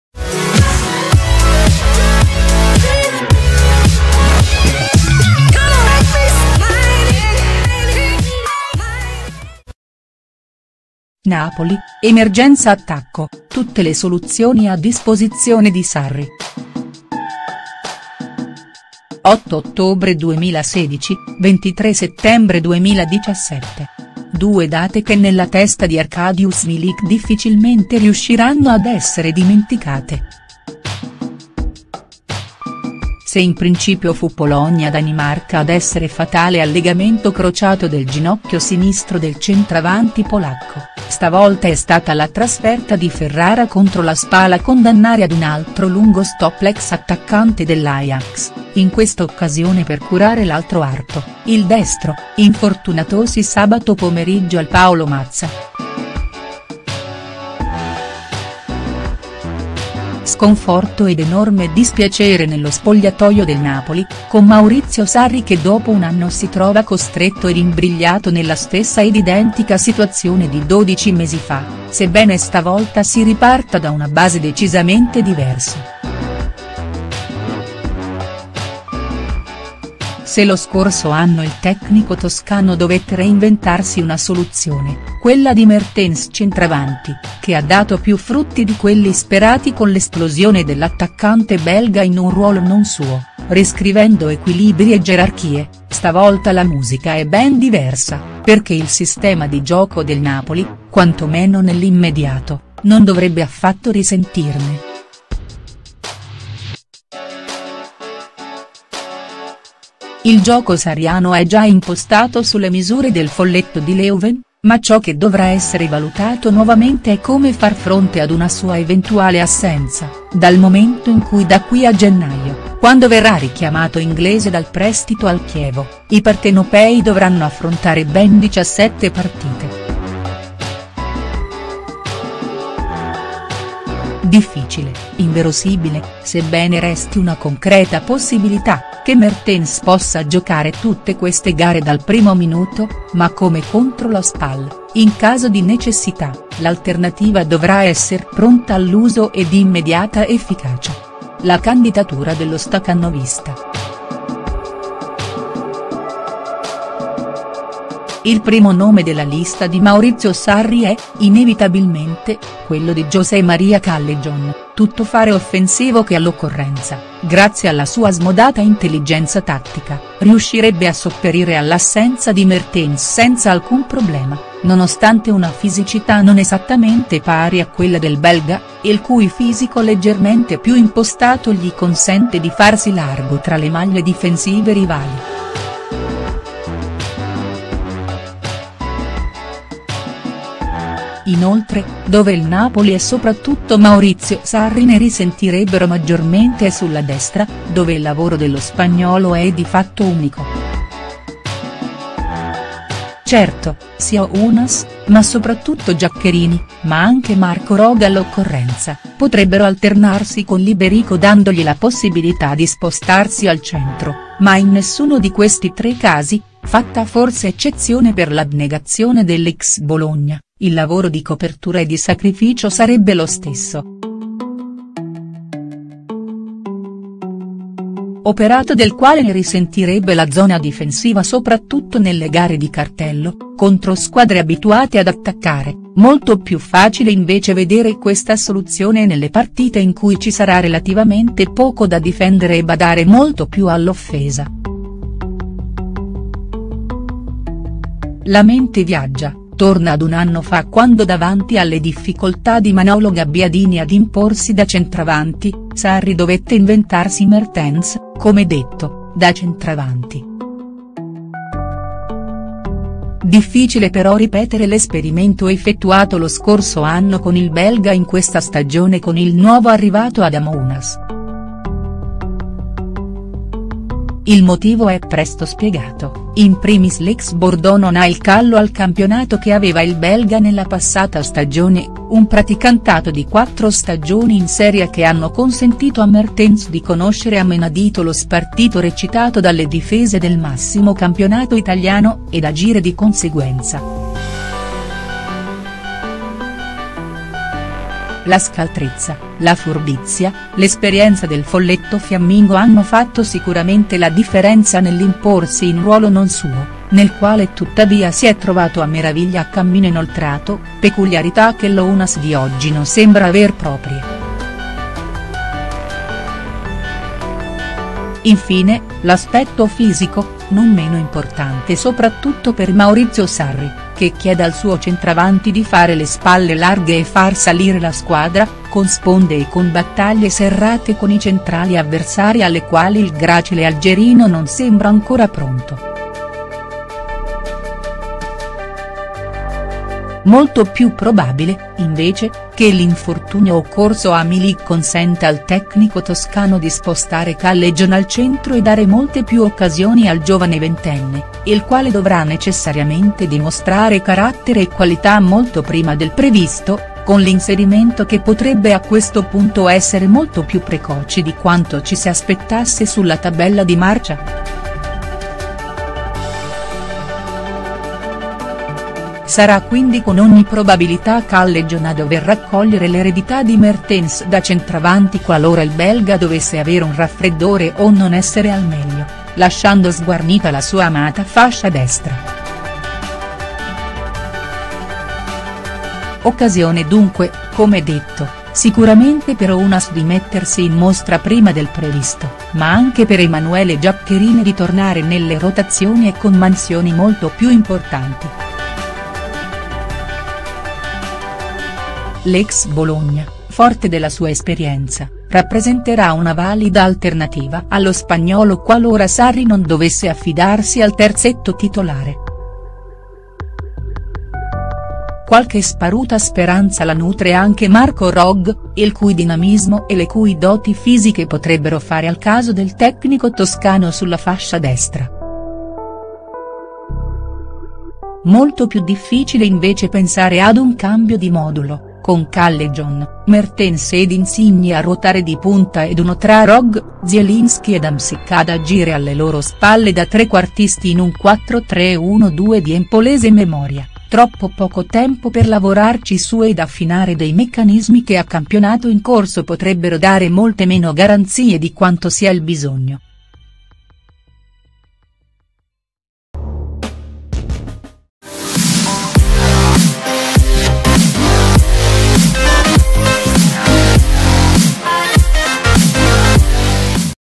Napoli, emergenza attacco: tutte le soluzioni a disposizione di Sarri.HD
Con Milik fuori dai giochi per quattro - o forse più - mesi, il tecnico toscano deve tornare a far fronte all'emergenza offensiva che lo scorso anno lo "costrinse" ad inventarsi Mertens centravanti. Ed ora, da dove ripartire?